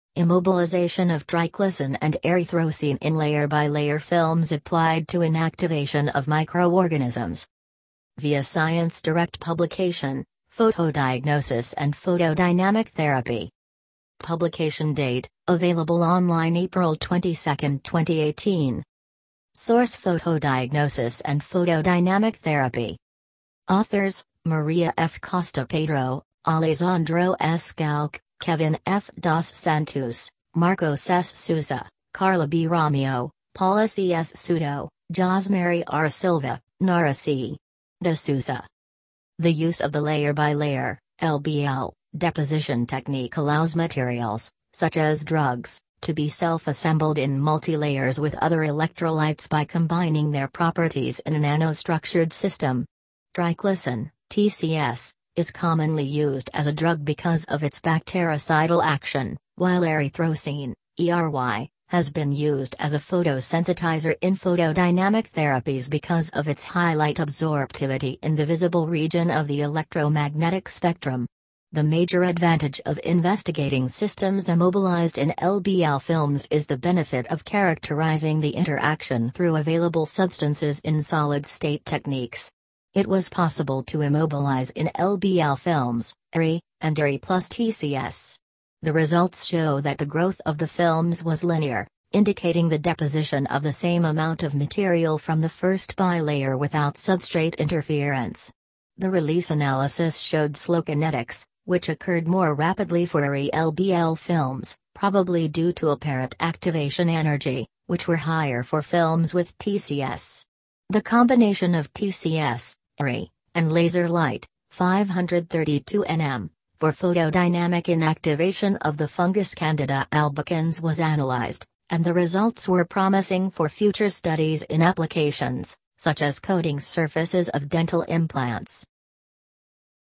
Immobilization of triclosan and erythrosine in layer-by-layer films applied to inactivation of micro
Immobilization of triclosan and erythrosine in layer-by-layer films applied to inactivation of microorganisms
via ScienceDirect Publication: Photodiagnosis and Photodynamic Therapy
Publication date: Available online 22 April 2018
Source:Photodiagnosis and Photodynamic Therapy
Author(s): Maria F. Costa Pedro, Alessandro S. Kalck, Kevin F. dos Santos, Marcos S. Sousa, Karla B. Romio, Paula C.S. Souto, Josmary R. Silva, Nara C. de Souza
The use of the layer-by-layer (LbL) deposition technique allows materials, such as drugs, to be self-assembled in multilayers with other electrolytes by combining their properties in a nanostructured system. Triclosan (TCS) is commonly used as a drug because of its bactericidal action, while erythrosine (ERY) has been used as a photosensitizer in photodynamic therapies because of its high light absorptivity in the visible region of the electromagnetic spectrum. The major advantage of investigating systems immobilized in LbL films is the benefit of characterizing the interaction through available substances in solid state techniques. It was possible to immobilize in LbL films, ERY, and ERY + TCS. The results show that the growth of the films was linear, indicating the deposition of the same amount of material from the first bilayer without substrate interference. The release analysis showed slow kinetics, which occurred more rapidly for ERY LbL films, probably due to apparent activation energy, which were higher for films with TCS. The combination of TCS, ERY, and laser light (532 nm) for photodynamic inactivation of the fungus Candida albicans was analyzed, and the results were promising for future studies in applications, such as coating surfaces of dental implants.